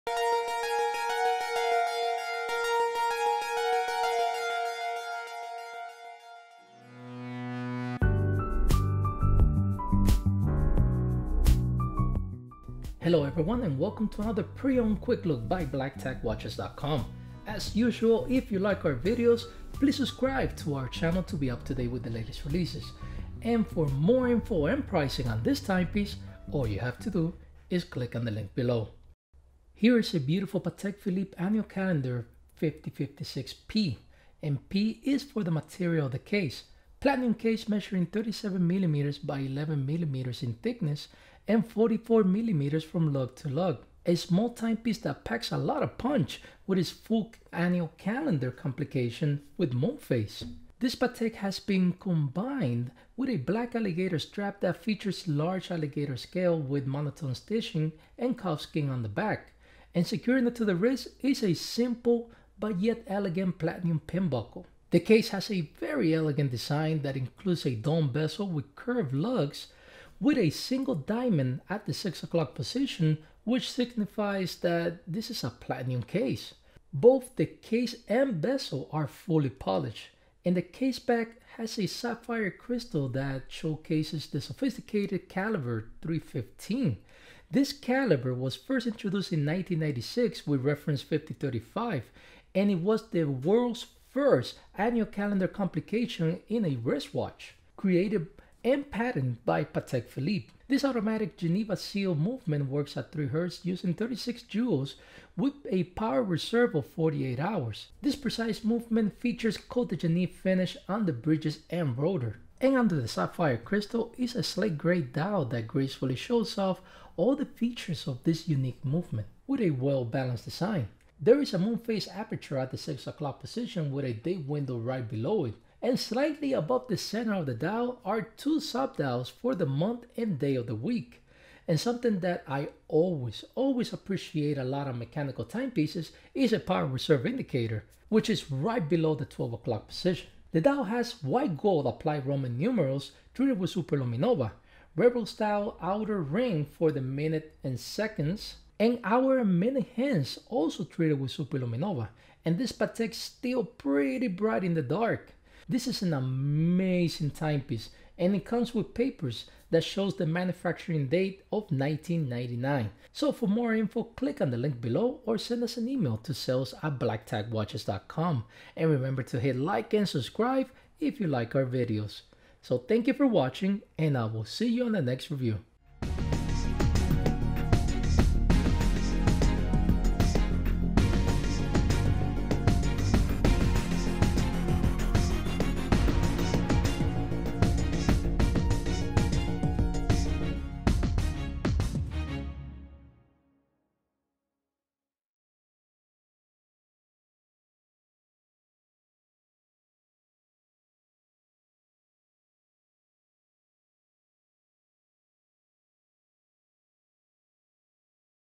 Patek Philippe Complications Annual Calendar Moon Phase 5056P
Pre-owned Patek Philippe Complications Annual Calendar Moon Phase 5056P.
37 mm platinum case. Grey dial features month sub-dial at 3 o’clock, moon phase indicator and date aperture at 6 o’clock, day of the week sub-dial at 9 o’clock, power reserve of 12 o’clock. Automatic Swiss Made Caliber 315 S IRM QA LU, features 37 jewels. Power reserve of 48 hours. Stamped with the seal of Geneva. Black new alligator leather strap with white gold deployment buckle. BTW travel case and Extract from Archives, dated 1999.